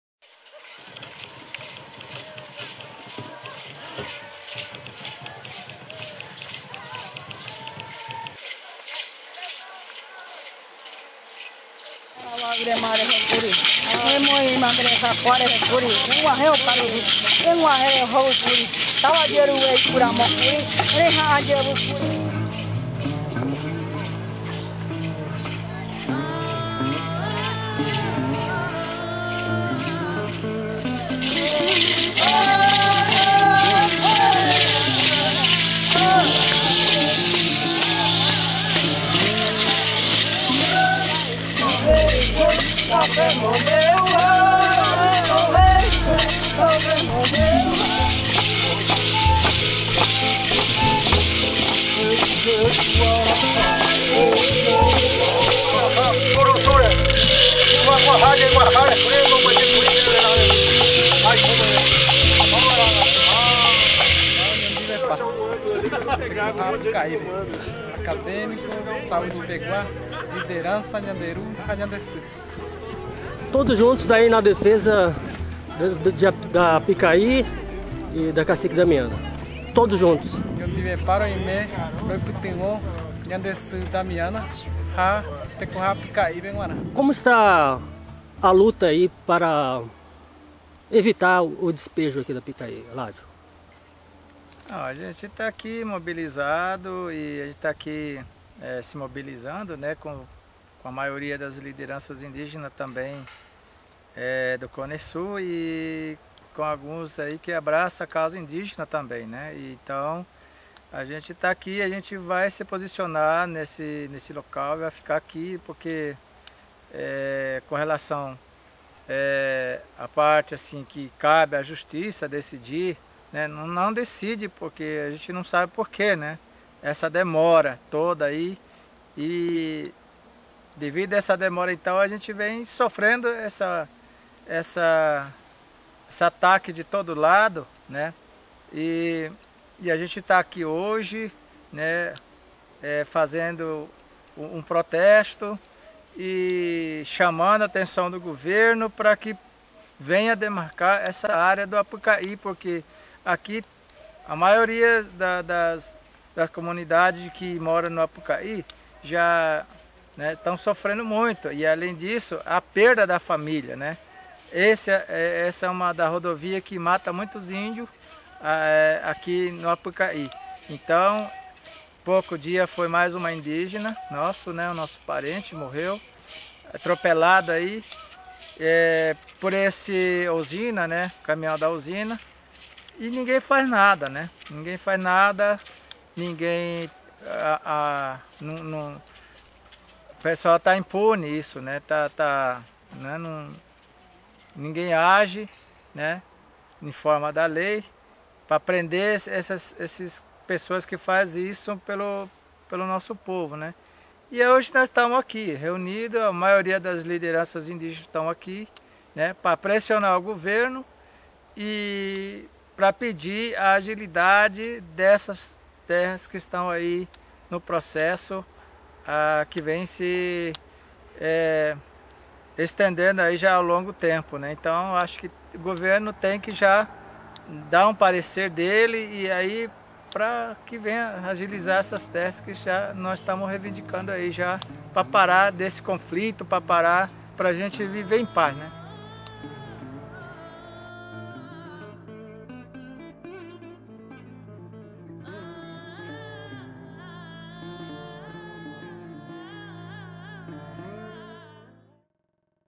GUARANI KAIOWÁ : APYKA'I RESISTE!
No Mato Grosso do Sul o povo Guarani Kaiowá   luta bravamente contra o agronegócio e o Governo Dilma que oPTou pelo latifúndio. O que querem esse povo? Apenas viver em paz nas terras que pertenciam aos seus antepassados.